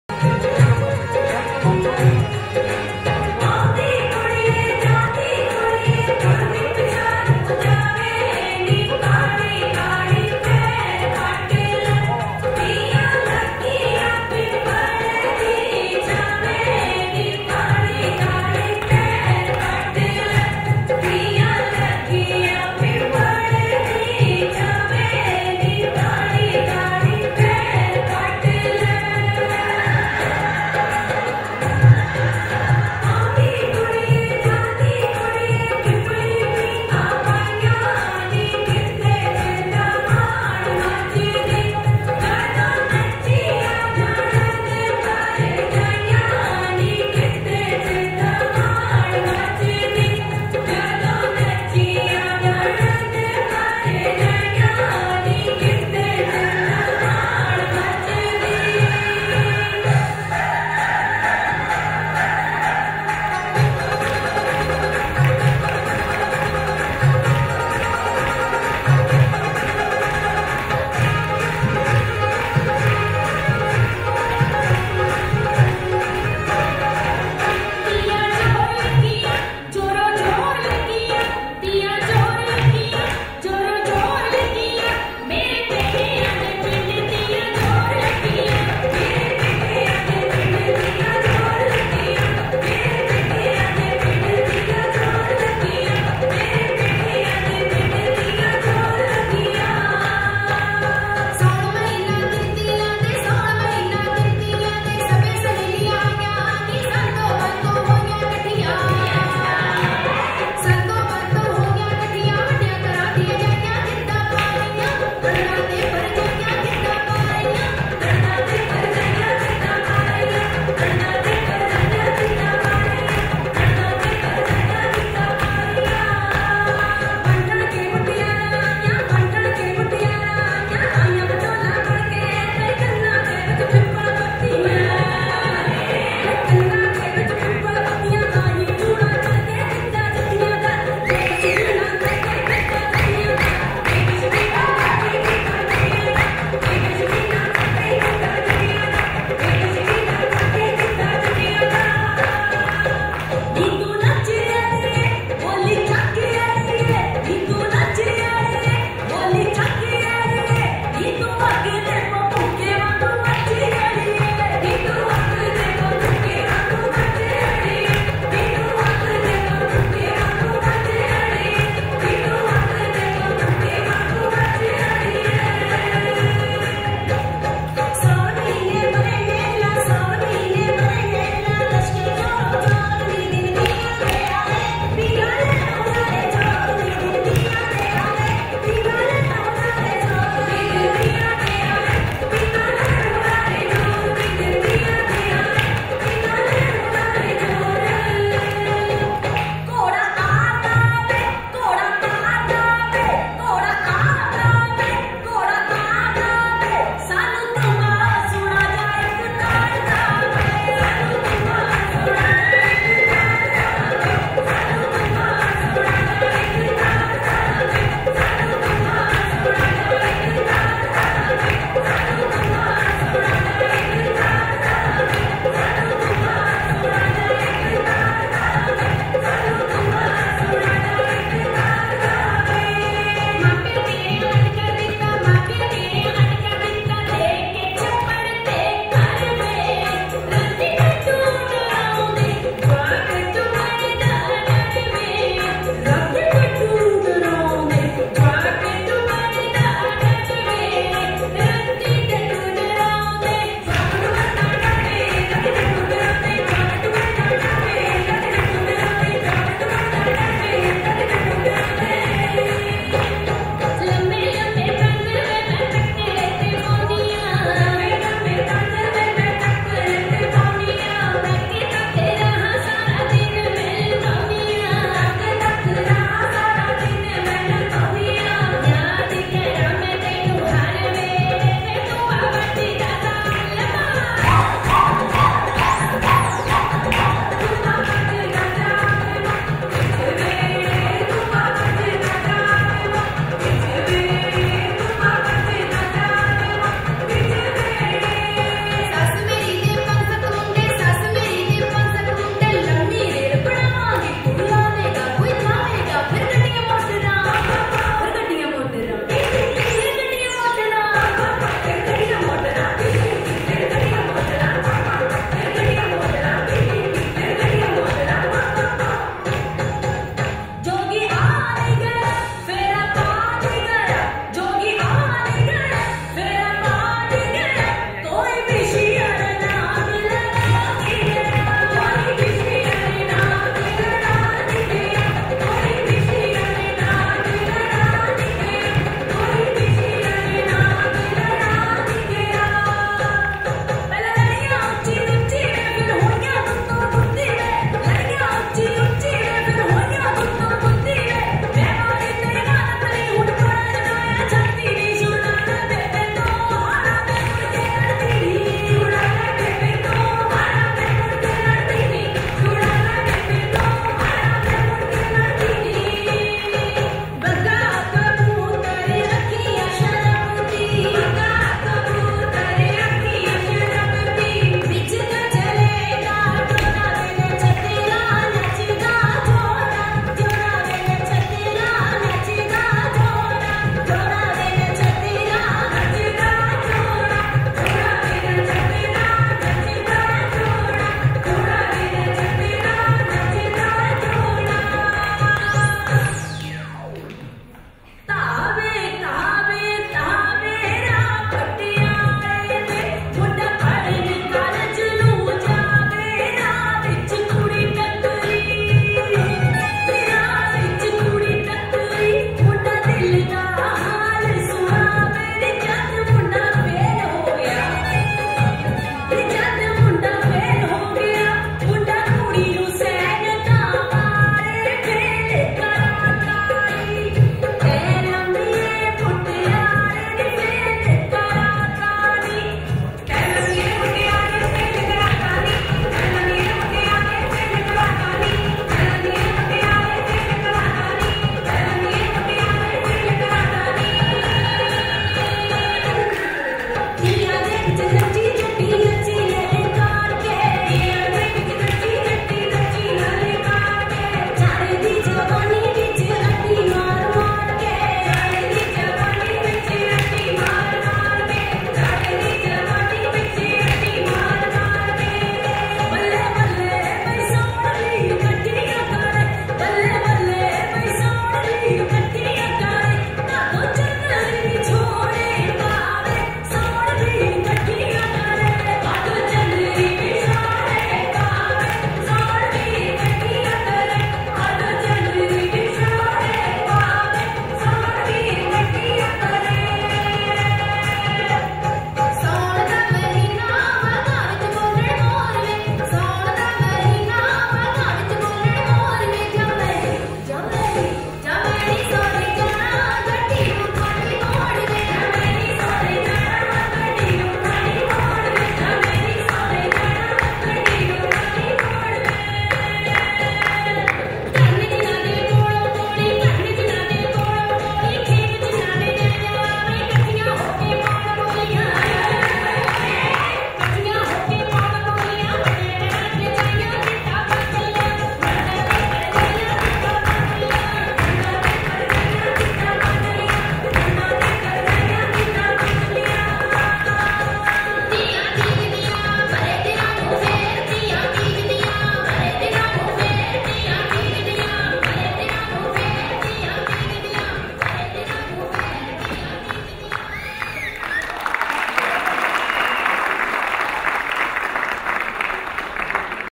Gidha Miss Australia Punjaban 2010 Giddha Round
punjabancontest 154 Gidha Round of Miss Australia Punjaban 2010-11 held on 27th September 2010 at RIMT University Melbourne Australia Organised by PHO Melbourne associate with Sabhyacharak Sathh Panjab India . Chairman Director Jasmer SIngh Dhatt